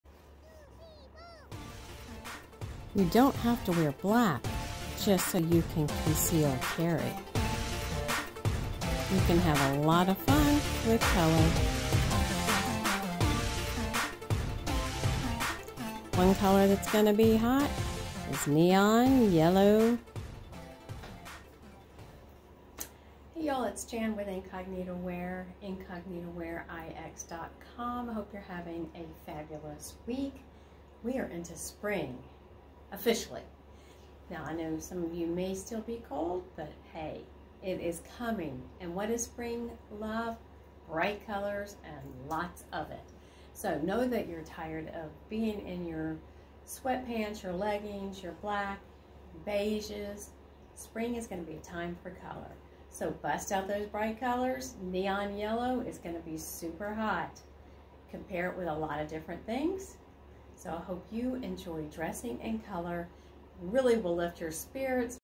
FashionFriday concealed carry in Color!
Colors, color, and more color!
Flowers are bursting with colors this time of year! Aren't you ready-to-get out-of winter, dark, drap clothes??
Bust out in yellow for the latest trend!